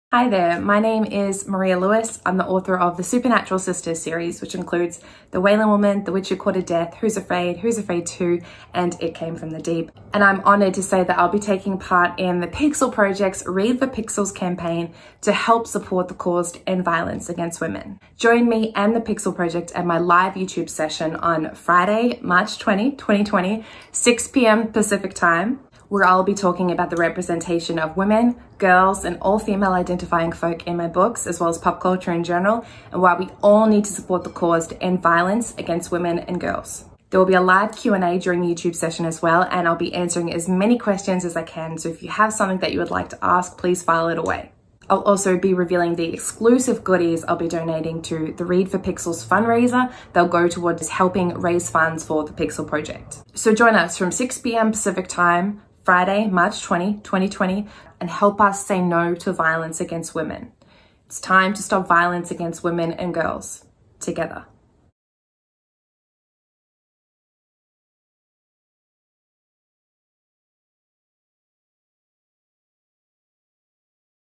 Read For Pixels 2020: Maria Lewis - Coming Soon on YouTube Live
Maria Lewis - award-winning bestselling author of the SUPERNATURAL SISTERS series - will be taking part in The Pixel Project's sixth annual International Women's Day Edition of the “Read For Pixels” campaign featuring live YouTube Readings+Q&A sessions with award-winning bestselling authors in support of The Pixel Project's work to end violence against women

It's time to stop violence against women. Together.

Producer: Regina Yau
Video Editor: Bret Primack